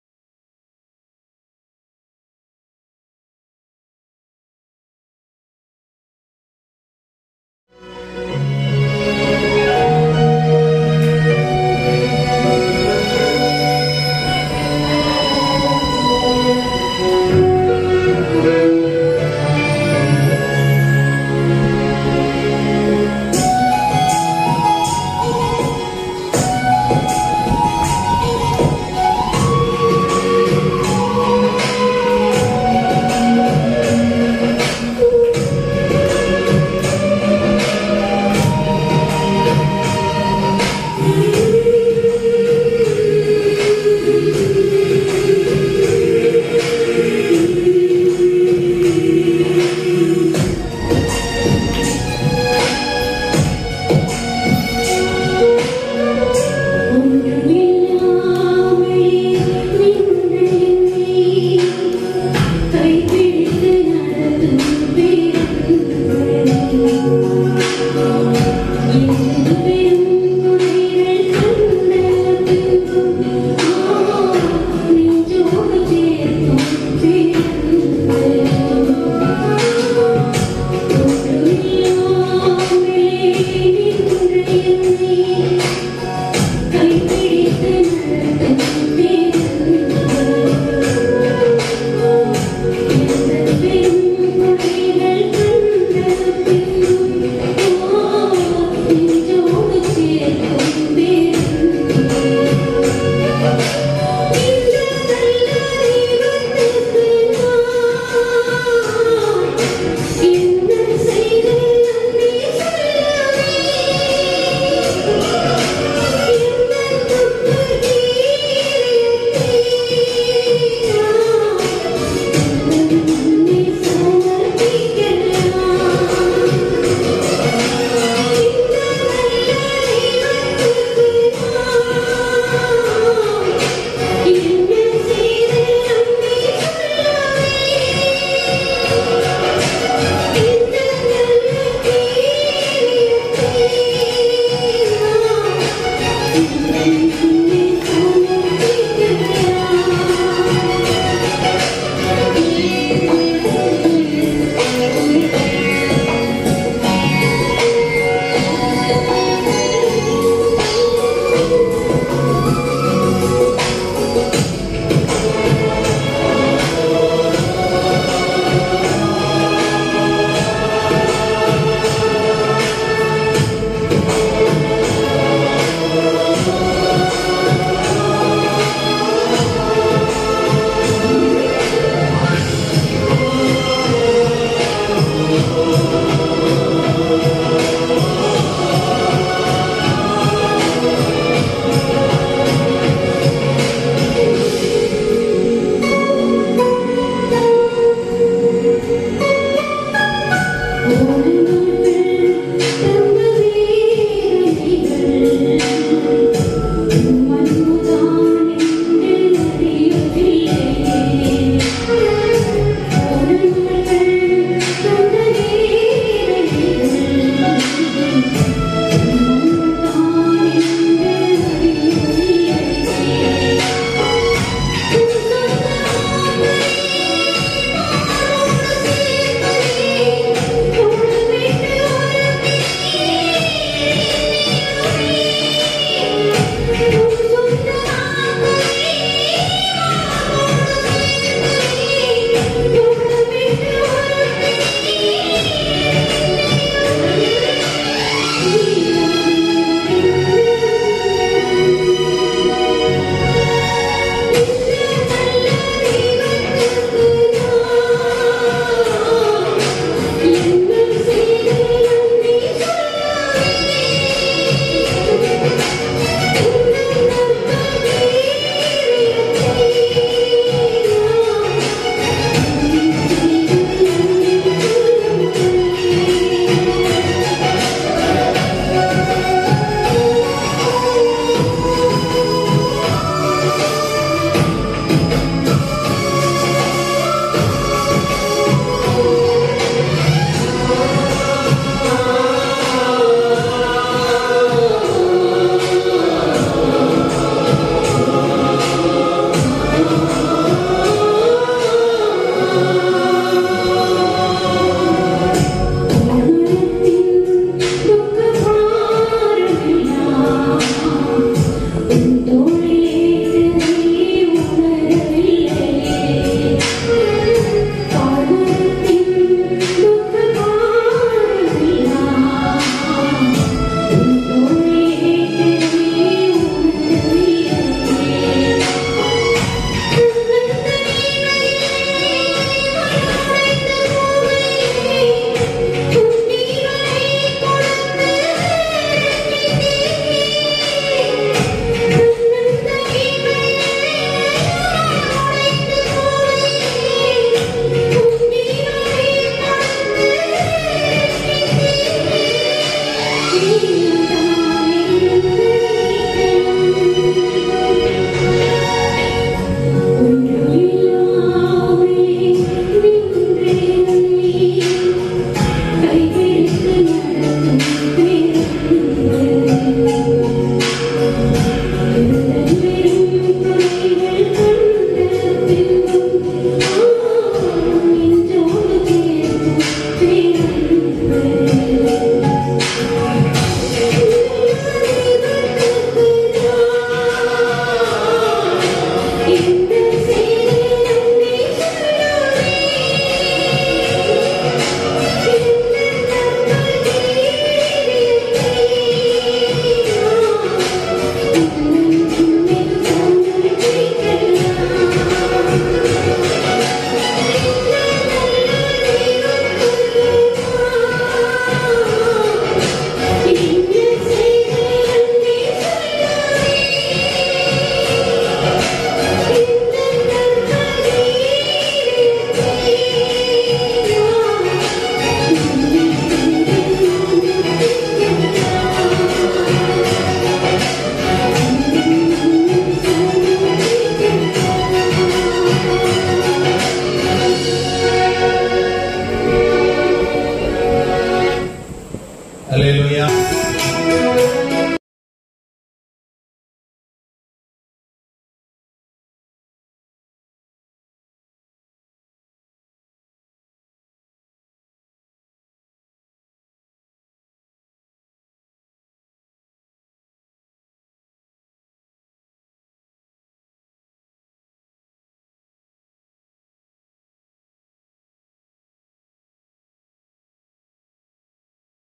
Super hit song - Sis.லீலா ஜோஸ்
இந்த பாடலின் மூலம் கர்த்தர் உங்களுடன் இடைப்பட்டு உங்களை எல்லா கட்டுகளிலிருந்தும் விடுதலையாக்குவார்,ஆறுதல் கொடுப்பார் ஆமென்.